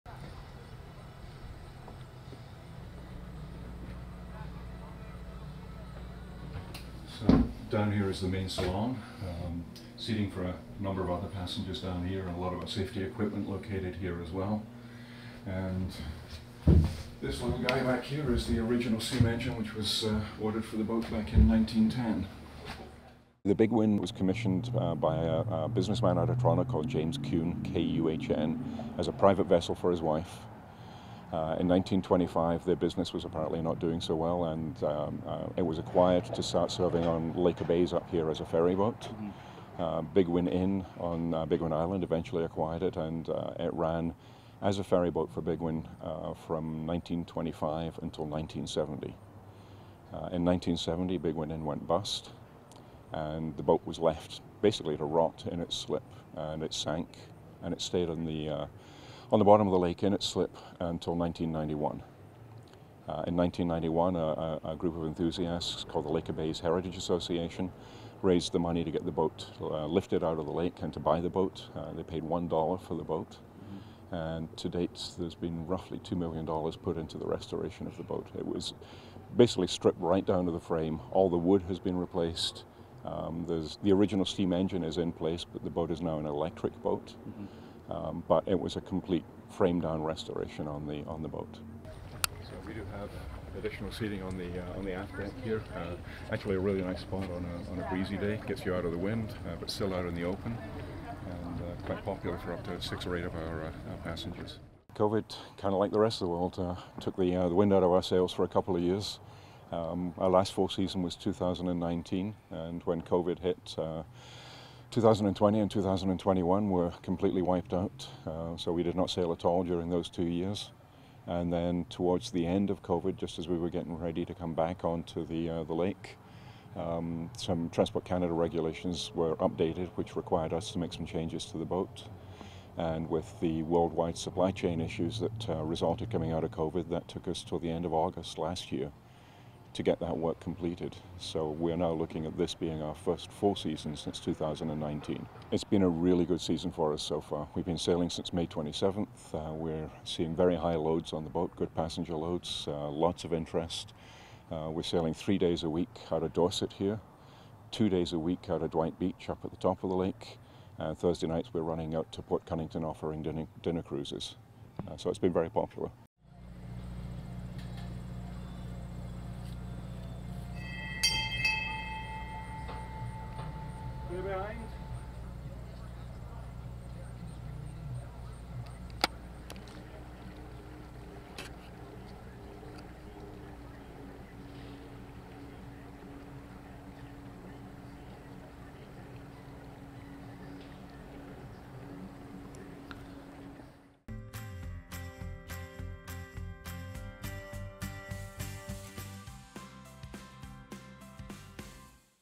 SS Bigwin is back for a full season following a cutdown on tours due to the pandemic
The SS Bigwin, a staple of the Dorset tourism industry, has gone through some challenges over COVID. But the little ship that could is no stranger to big challenges and as Captain Jim Speirs tells us, it has come back again better than ever.

To stay up-to-date on all things local go to YourTV.tv

SS Bigwin Discover Muskoka